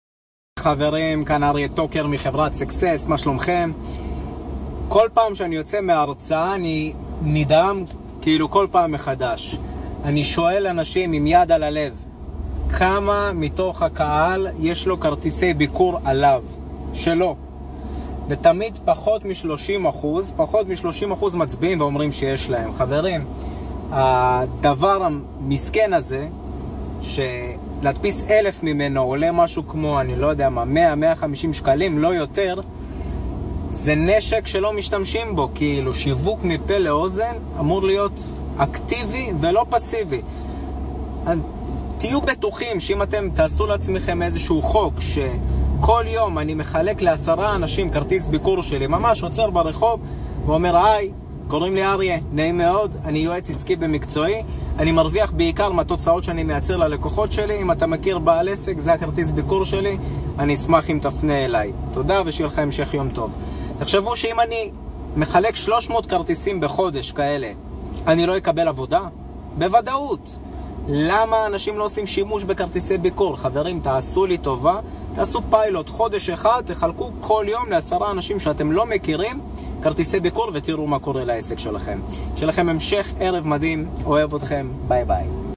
יש לכם כרטיסי ביקור בארנק?
האם יש לכם בארנק כרטיסי ביקור בכל זמן נתון?
איך כרטיסי ביקור בארנק יכולים להכניס לכם עבודה?
אריה טוקר, מחברת SUCCESS ייעוץ עסקי, עם התשובות.